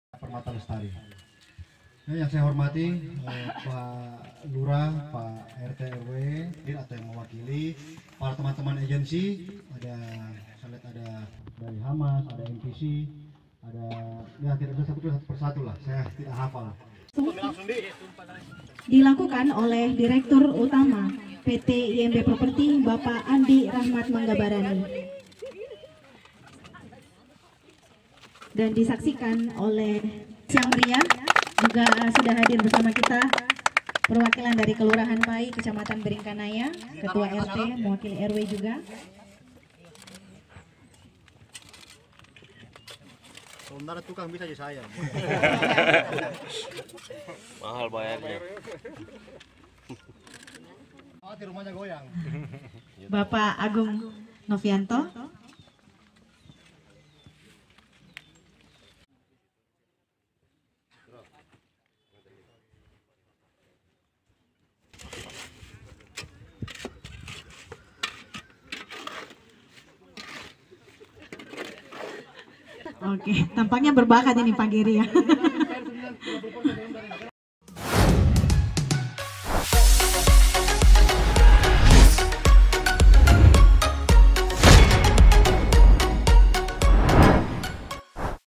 IMB Property Letakkan Batu Pertama Pembangunan Griya Permata Lestari
MAKASSAR - IMB Property meletakkan batu pertama pembangunan perumahan Griya Permata Lestari, di Jl Batu Tambung, Kelurahan Pai, Kecamatan Biringkanaya, Makassar, Kamis (25/7/2019).

Batu pertama diletakkan oleh Direktur Utama IMB Property, Andi Rahmat Manggabarani.

Peletakan disaksikan sejumlah mitra dari perbankan dan pemerintah setempat.

Andi Rahmat Manggabarai mengatakan, peletakan batu pertama ini menjadi titik awal IMB Property dalam pembangunan Griya Permata Lestari yang sudah di-launching satu bulan sebelumnya.

Andi Rahmat juga menjelaskan, alasan IMB Property membuat acara seremoni peletakan batu pertama.

IMB Property ingin menunjukkan sebagai pengembang profesional, sehingga para user tidak perlu khawatir.

"Mengapa kami buat seremonial, kami  ingin menunjukkan karena beberapa hari ini saya baca berita yang kurang mengenakkan di media, bahwa ada pengembang bawa kabur uang user di beberapa daerah," kata dia.

"Oleh karena itu kami ingin tunjukkan bahwa IMB Property bukan pengembang seperti itu, kami tunjukkan apa yang telah dibayarkan user dan dijual marketing betul-betul akan terwujud dan terealisasi sesuai janji kami, yaitu 18 bulan serah terima sejak tanggal akad kredit," tambahnya.

Berikut video peletakan batu pertama pembangunan perumahan Griya Permata Lestari.  (tribun-timur.com)